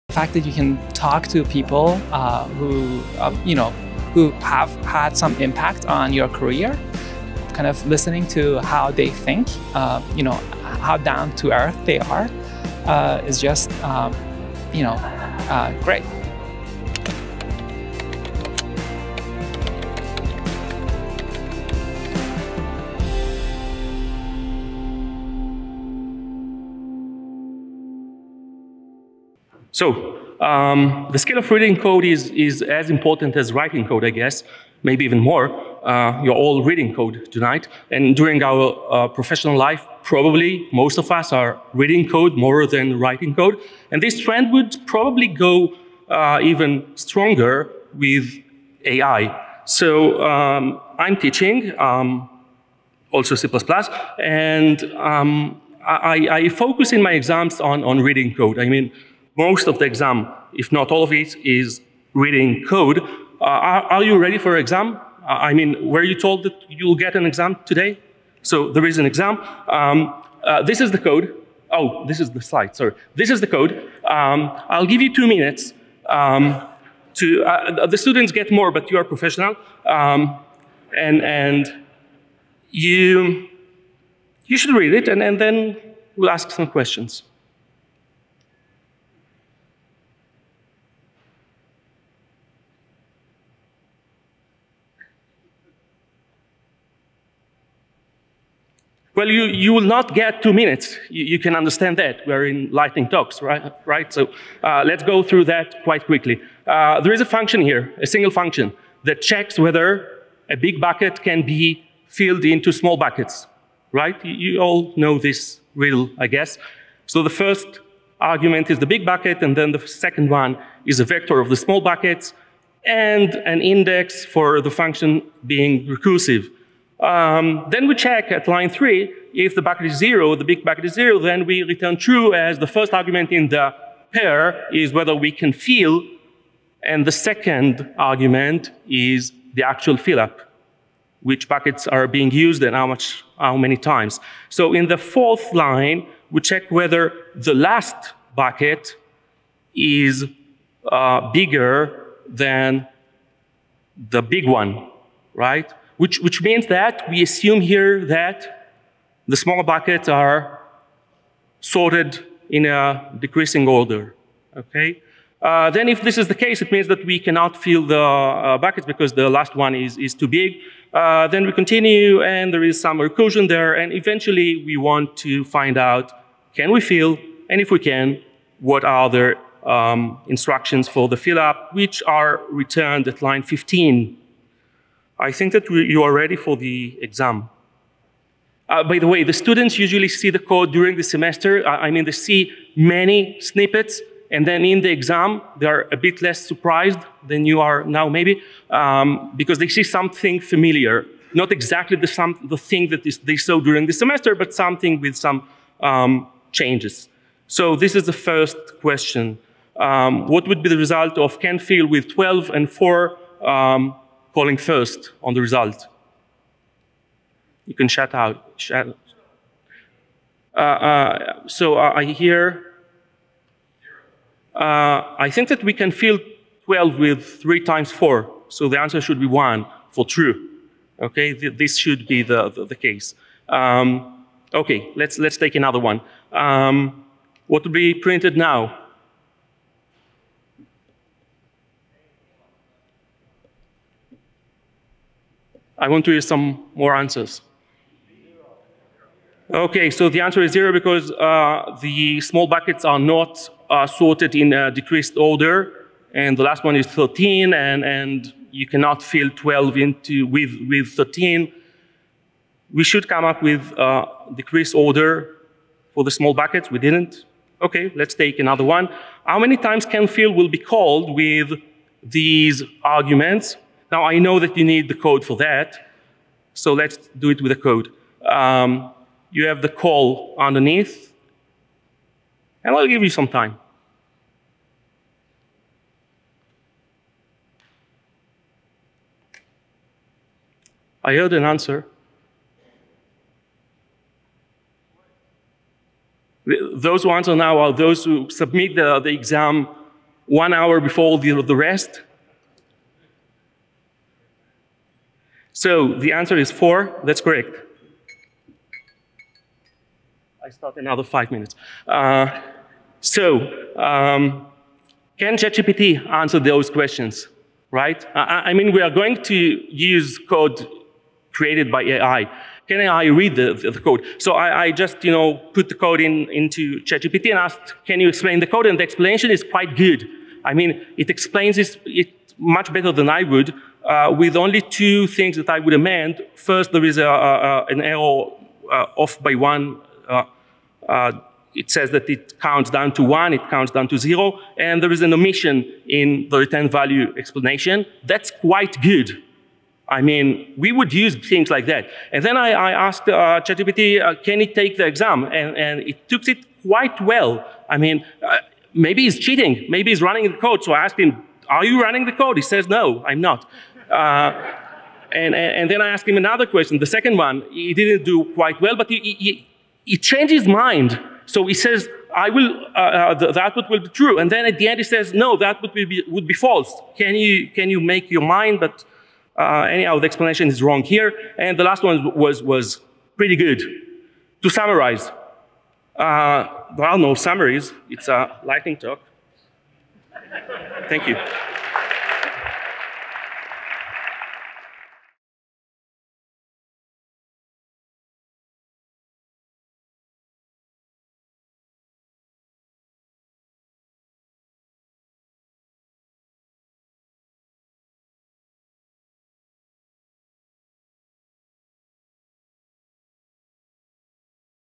Lightning Talk: Filling the Bucket: Reading Code, C++ Code Interviews & Exams - Amir Kirsh - CppCon
Lightning Talk: Filling the Bucket: Reading Code, C++ Code Interviews & Exams - Amir Kirsh - CppCon 2023

We are going to review and practice a reading code challenge. Reading code skills are quite important, maybe even more than writing code. So let's dive together into filling the bucket code reading challenge!

Amir Kirsh

Amir Kirsh is a C++ lecturer at the Academic College of Tel-Aviv-Yaffo and a visiting lecturer at Stony Brook University, previously the Chief Programmer at Comverse, after being CTO and VP R&D at a startup acquired by Comverse. He is also a co-organizer of the annual Core C++ conference and a member of the ISO C++ Israeli National Body.